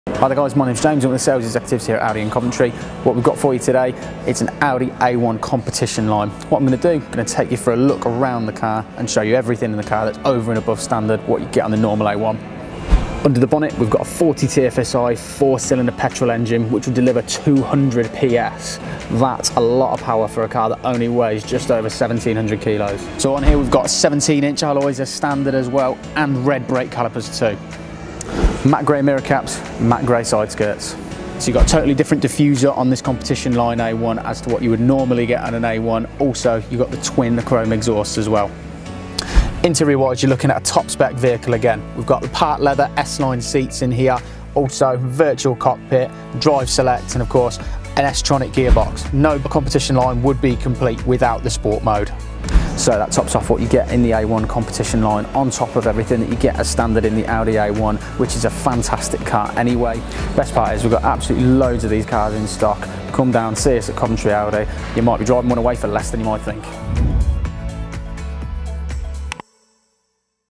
Audi A1 S-Line Competition | What's different about this new top spec A1?
The new Audi A1 Competition, is the new range topping version of the A1.  To show you just what is so special about this car, James has got hands on with this small but mighty Audi.